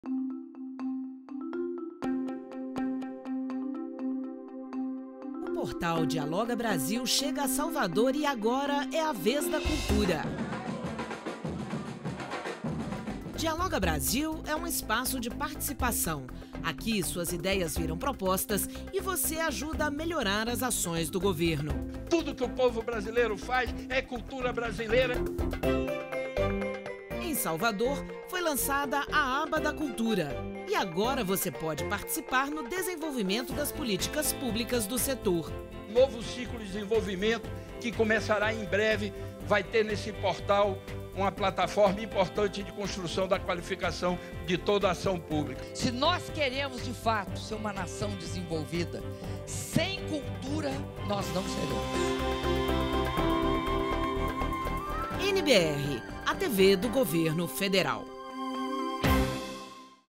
Plataforma Dialoga Brasil lança nova aba da Cultura
INTERPROGRAMAS - 25.08.15: Dialoga Brasil é um espaço digital de participação popular no planejamento das ações do Governo Federal. A nova aba da Cultura, lançada em Salvador (BA), agora possibilita a participação na formulação de políticas públicas no setor. Participe em dialoga.gov.br